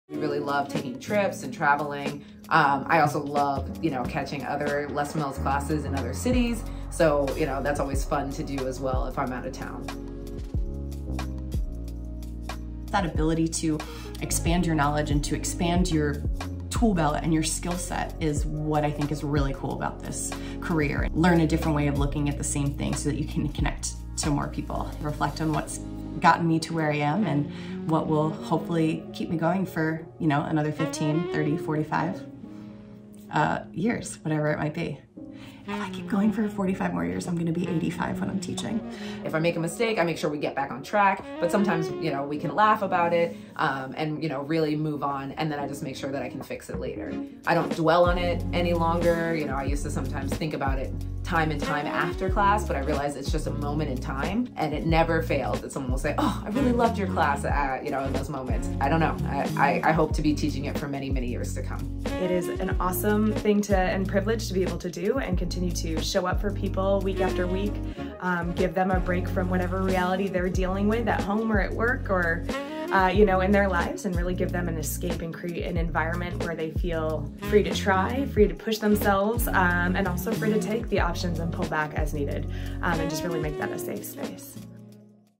ENJOYMENT | Instructors on Instructor Longevity
Veteran instructors share tips on fitness instructor enjoyment.